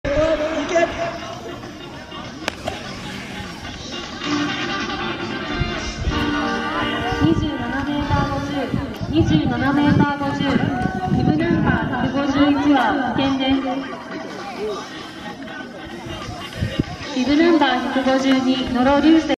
村山仁汰 選手 第31回飯山市サマー １本目 27.5メーター
秋津小６年のジンタ選手。１本目、小学生男子の部４位。
2023/06/25(日) 飯山シャンツェ